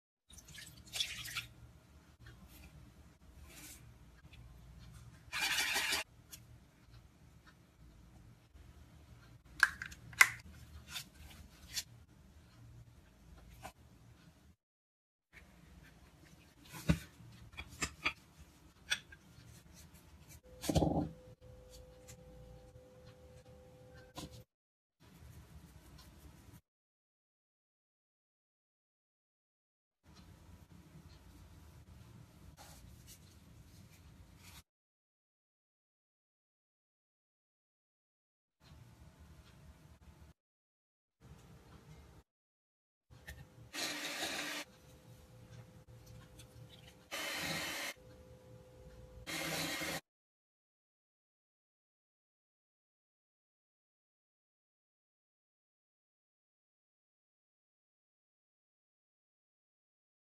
طريقة عمل سينابون هش وطري ولذيذ في المنزل 😋
طريقة عمل سينابون